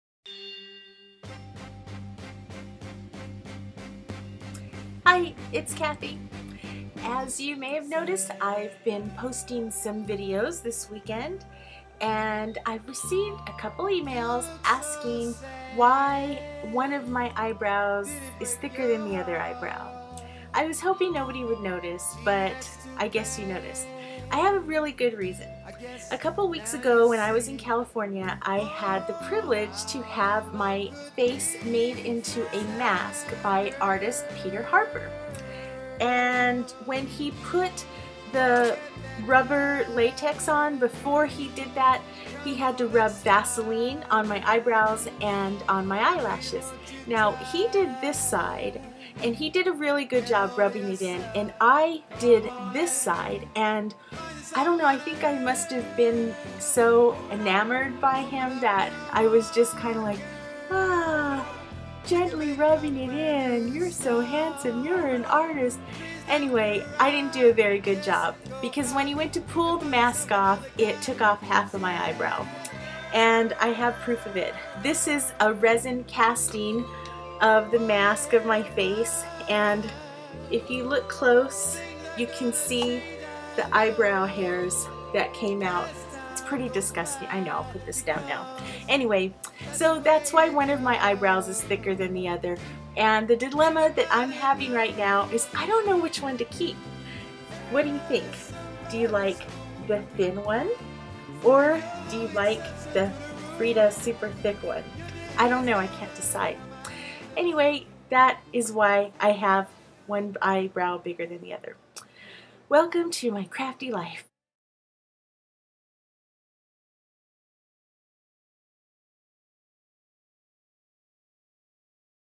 Eerie Eyebrows
The drama never stops! I share the story behind my one skinny, one thick eyebrow!